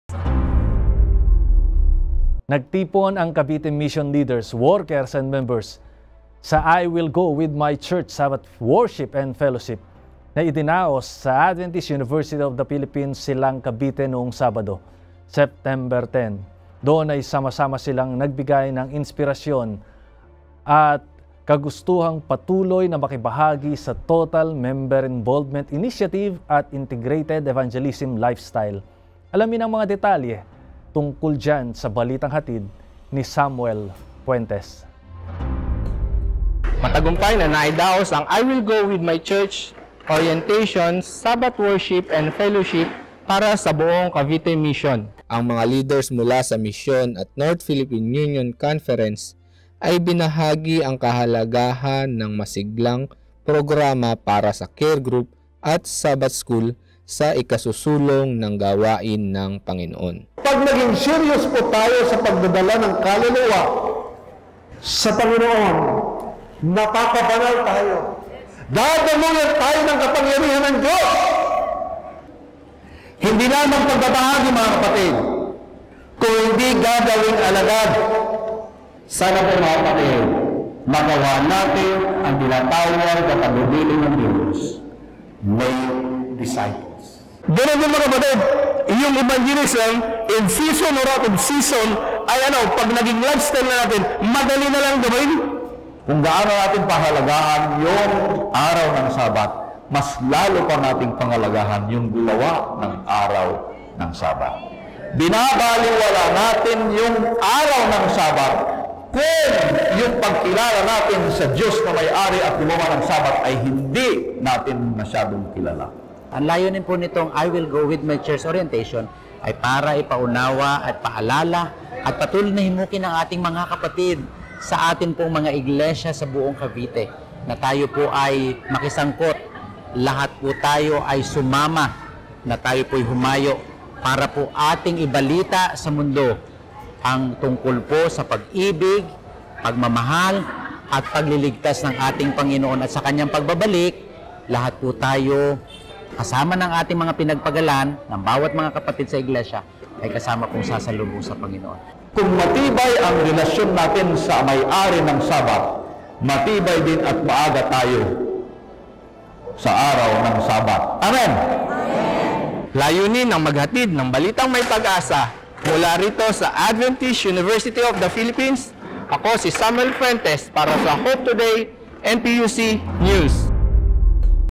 NDR-IEL Orientation nagdulot ng bagong sigla sa ministry ng CM leaders, members
Nagtipon ang Cavite Mission leaders, workers and members sa "I Will Go with My Church" Sabbath worship and fellowship na idinaos sa AUP, Silang, Cavite.

Doon ay sama-sama silang nabigyan ng inspirasyon at kagustuhang patuloy na makibahagi sa Total Member Involvement initiative at Integrated Evagelism Lifestyle.

Alamin ang mga detalye tungkol dyan sa balitang hatid ni Samuel Fuentes…